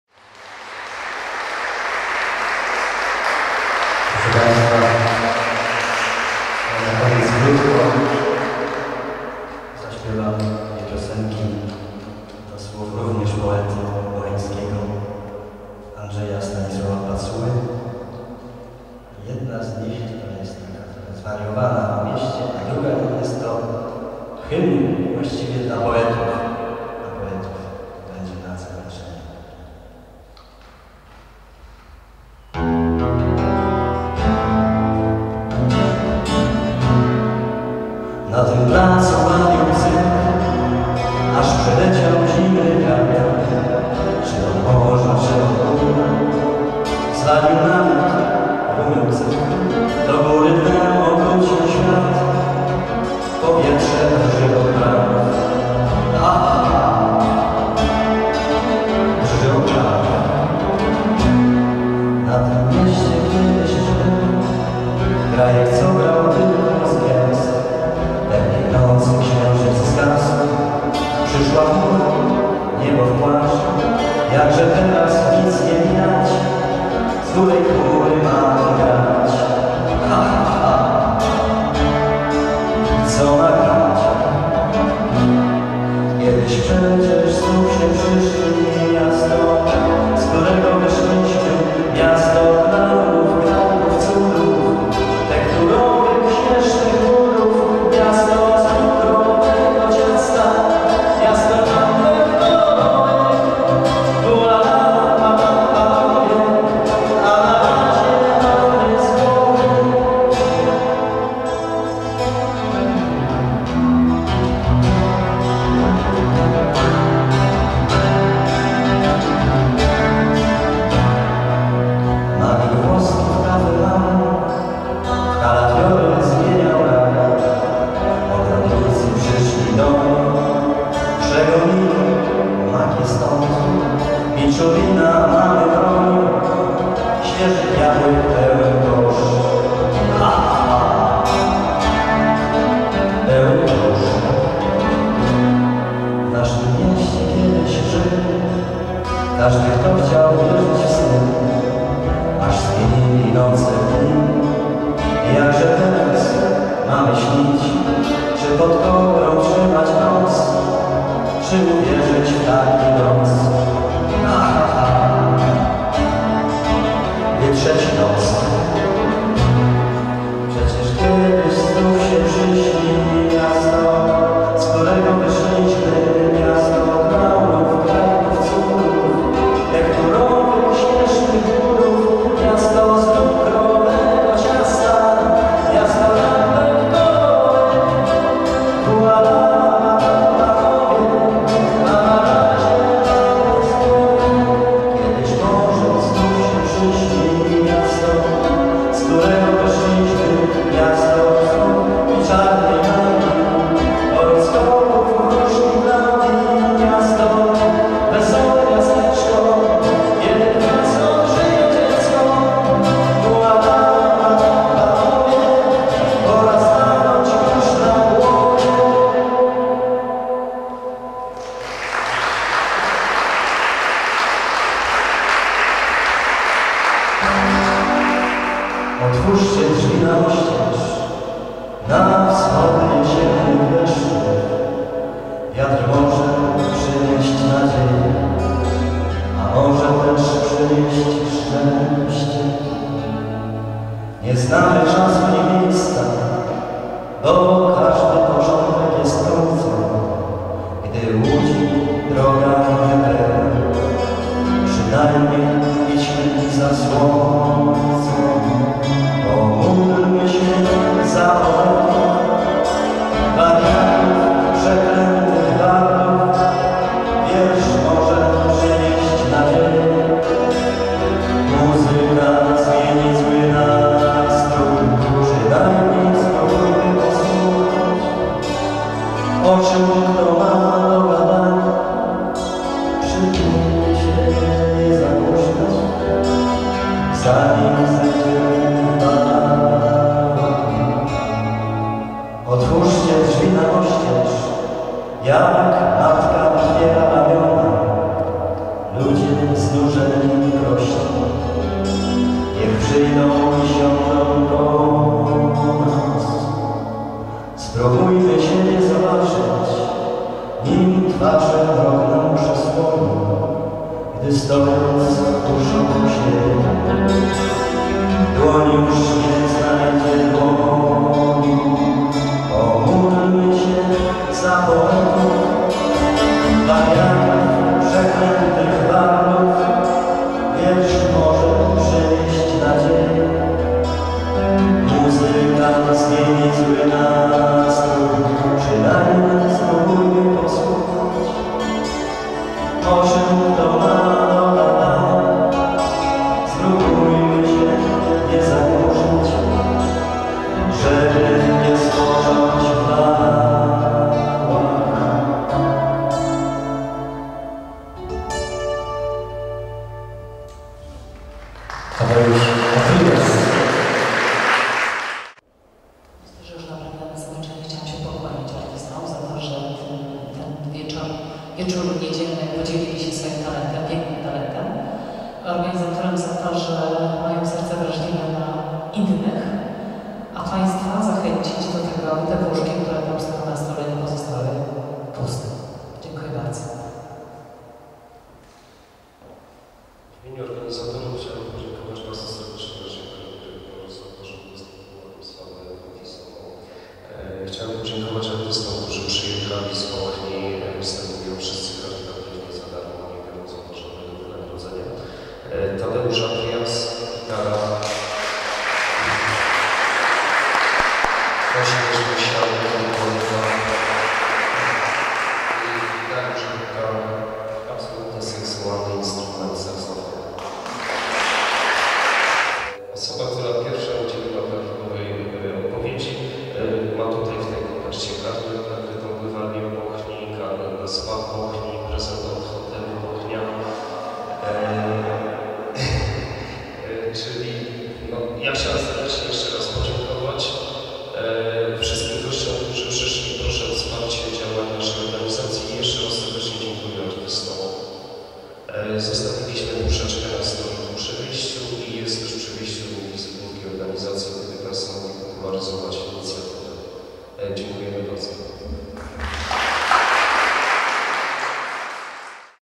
Tadeusz Apryjas
Występ Tadeusza Apryjasa na charytatywnej imprezie zorganizowanej przez fundację Auxilium dzięki uprzejmości Dyrekcji Zespołu Szkół Ponadgimnazjalnych nr 1 w Brzesku,
w dniu 30 czerwca 2013 r.